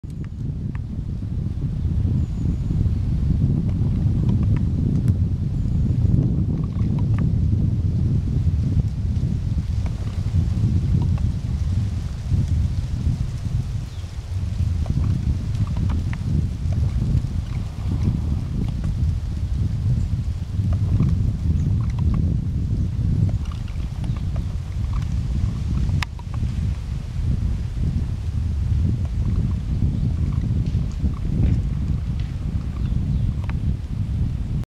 Fauna Lasu Rędzińskiego
Kogo można spotkać jadąc wałem przy ujściu Widawy do Odry (od strony Świniar)